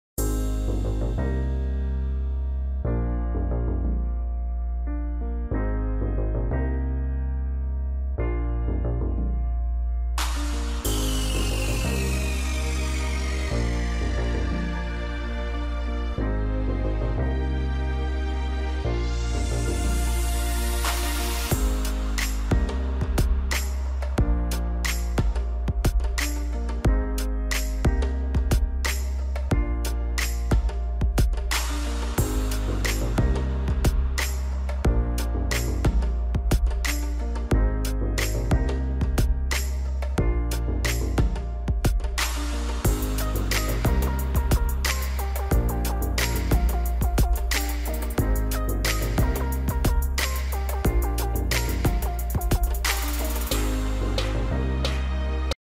Oddly satisfying video - Horse Drawing with Pencil
Draw a Horse with pencil. shading